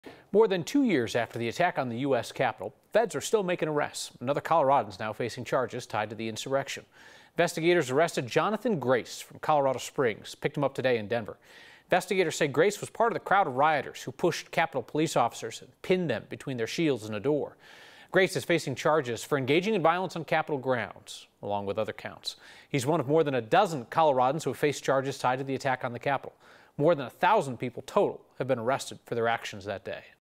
Another Coloradan arrested for alleged role in Jan. 6 insurrection
Another Coloradan is facing charges tied to the insurrection.

9NEWS (KUSA) is located in Denver, Colorado.